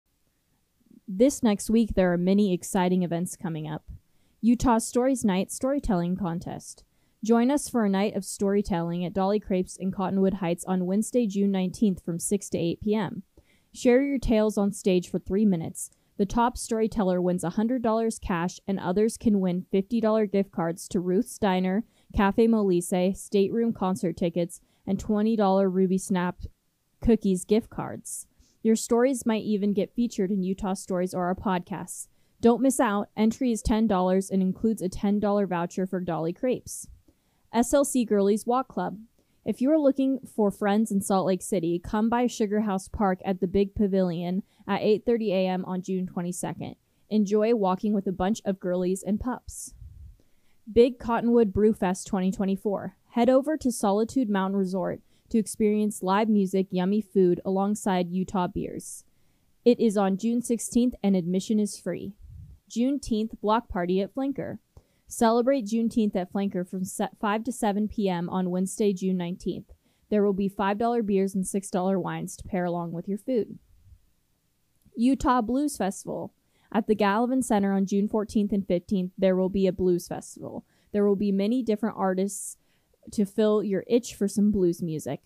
Events Coming Up in Utah
This next week there are many exciting events coming up. Utah Stories Night: Storytelling Contest Join us for a night of storytelling at Dali Crepes in Cottonwood Heights on Wednesday, June 19th, from 6 to 8 pm! Share your tales on stage for three minutes. The top storyteller wins $100 cash, and others can win $50 gift cards to Ruth's Diner, Caffe Molise, State Room concert tickets, and $20 RubySnap Cookies gift cards. Your stories might even get featured in Utah Stories or our podcasts. Don't miss out! Entry is $10 and includes a $10 voucher for Dali Crepes. SLC Girlies Walk Club If you are a girl looking for friends in Salt Lake City, come by Sugar House Park at the big pavilion at 8:30 a.m on June 22nd. Enjoy walking with a bunch of girlies and pups. Big Cottonwood Brew Fest 2024. Head over to Solitude Mountain Resort to experience live music, yummy food alongside Utah beers. It is on June 16th and admission is free. Juneteenth Block Party at Flanker Celebrate Juneteenth at Flanker from 5-7 p.m. on Wednesday June 19th. There will be $5 beers and $6 wines to pair along with your food. Utah Blues Festival At the Gallivan Center on June 14th and 15th there will be a Blues Festival. There will be many different blues artists to fill your itch for some blues music.

Go to utahstories.com to read the full story.